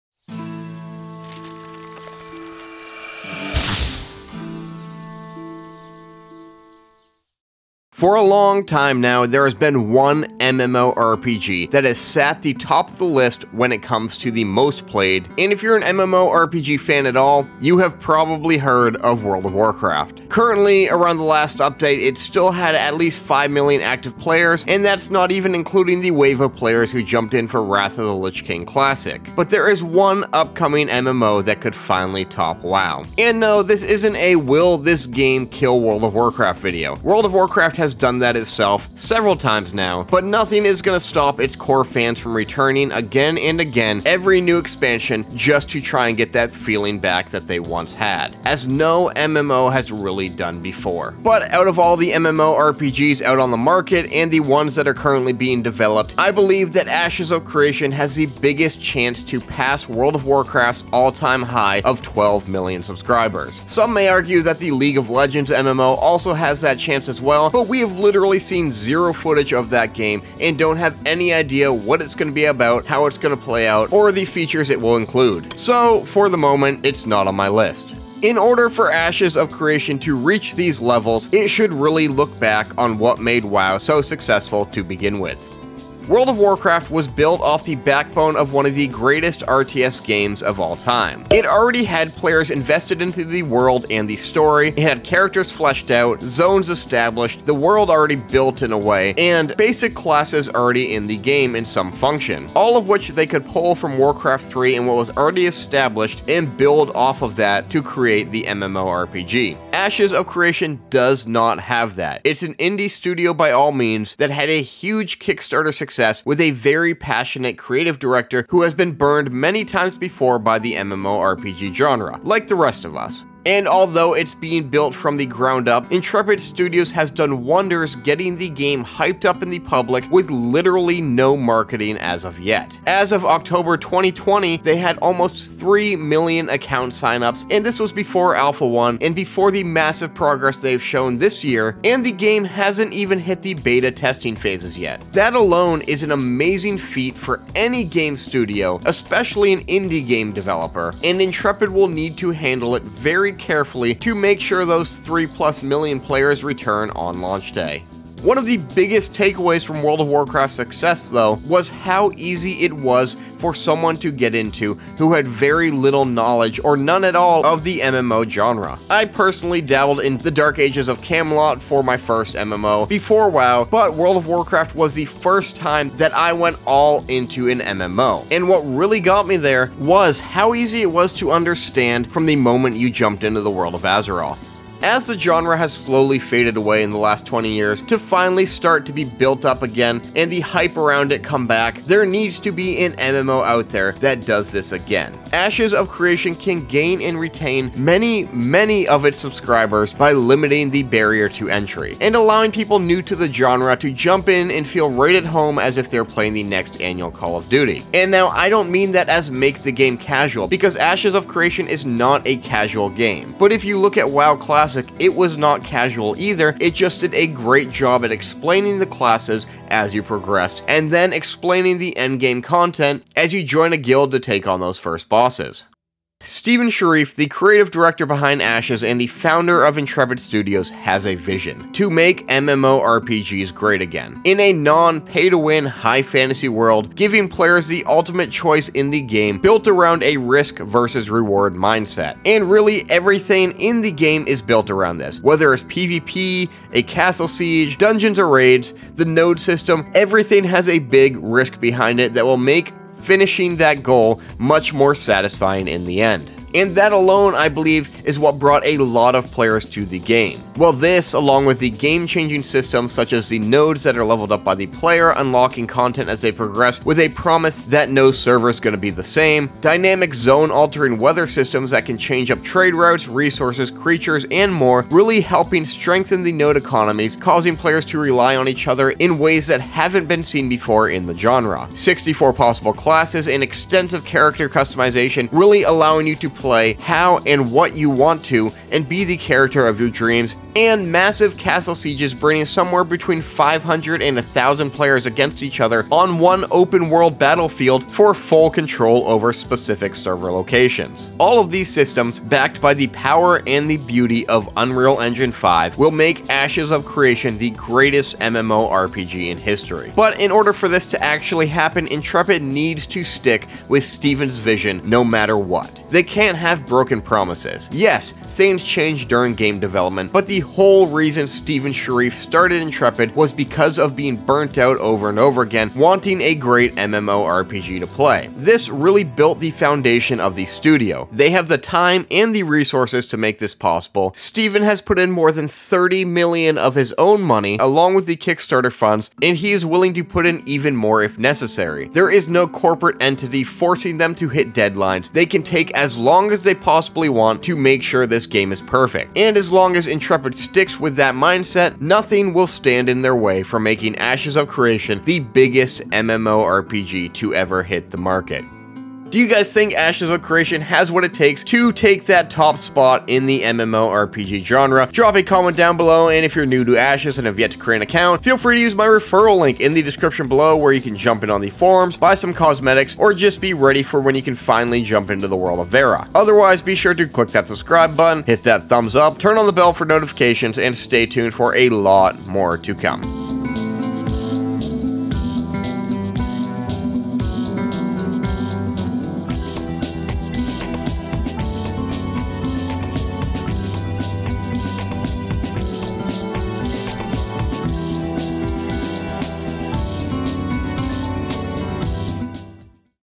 Can Anything Stop Ashes of Creation?
MMORPGS the last few years are easy to come by. From World of Warcraft, to Final Fantasy, Elder Scrolls Online, Lost Ark, and many more, all fighting for that place in our hearts to be THE next MMORPG. But I believe that Ashes of Creation is the only one who can fill that role